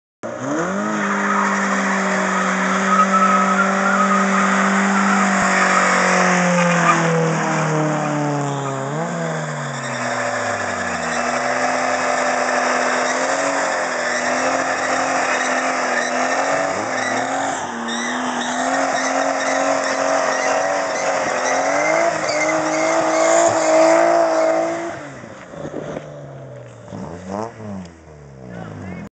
honda vs. nissan burnout
my buddys 2.2L accoord with cold air intake and exhaust vs. my 1990 240sx with rb20det running 12psi just removing miles from the tires =P